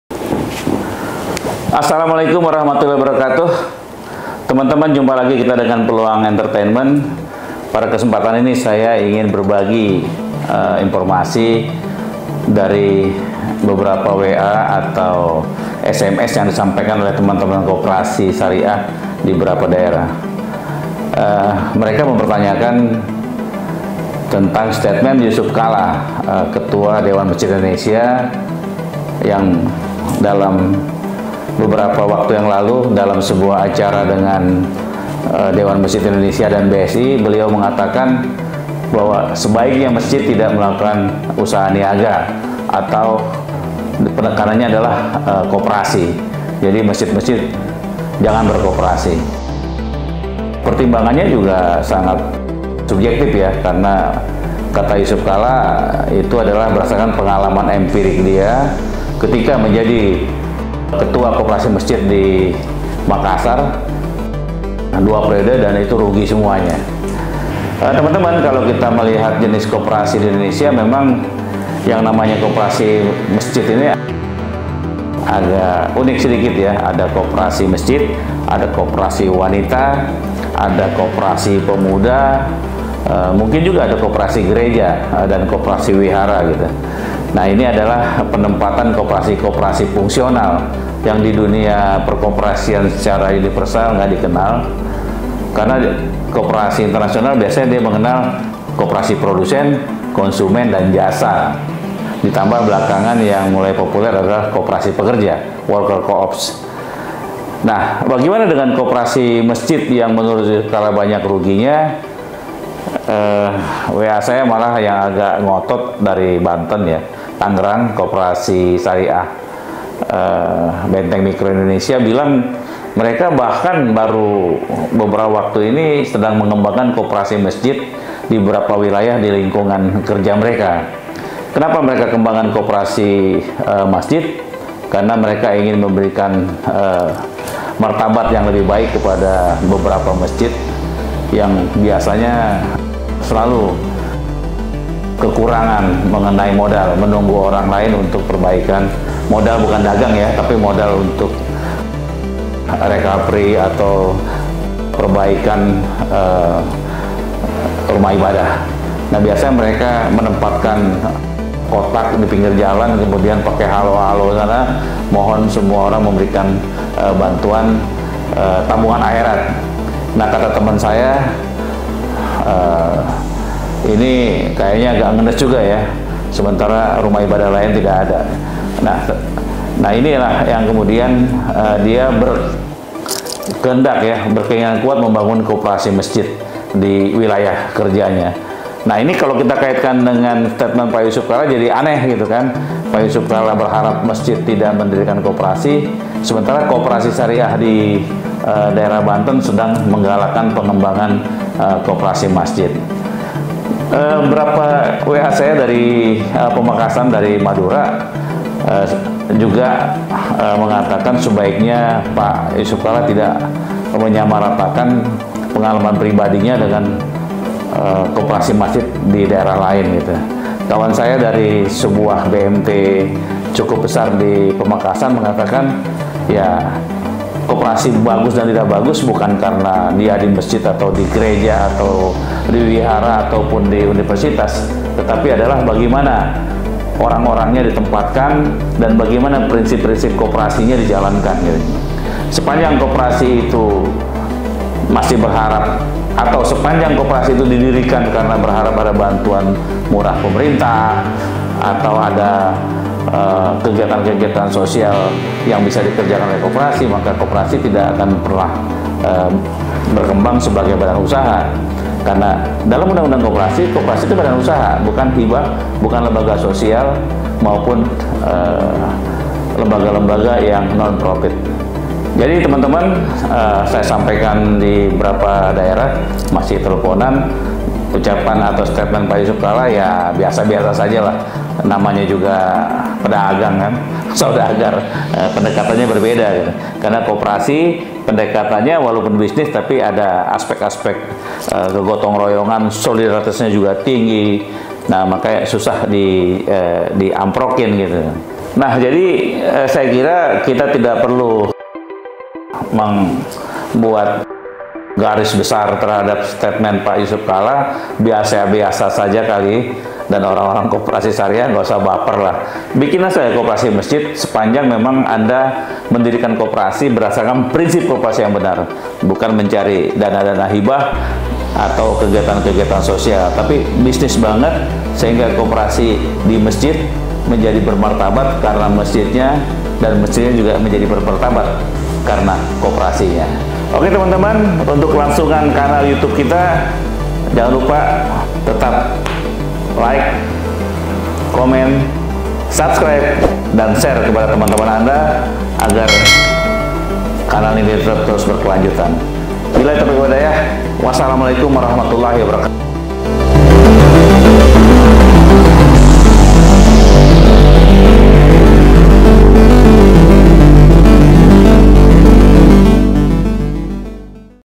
DILARANG BERKOPERASI DI MASJID
Jusuf Kalla minta Masjid tak bikin koperasi atau unit usaha, berkaca pengalaman beliau sebelumnya setiap masjid membuat koperasi atau unit usaha selalu berkahir pada kerugian. Hal itu disampaikan Jusuf Kalla dalam penandatanganan nota kesepahaman antara Bank Syariah Indonesia dan Dewan Masjid Indonesia di Kantor BSI. Akibat dari pernyataan tersebut, timbul pertentangan dari sejumlah Koperasi Syariah di Indonesia yang melakukan pengembangan Koperasi di Masjid. Menurut anda, perlukah adanya pembentukan Koperasi di Masjid, jawab di kolom komentar ya.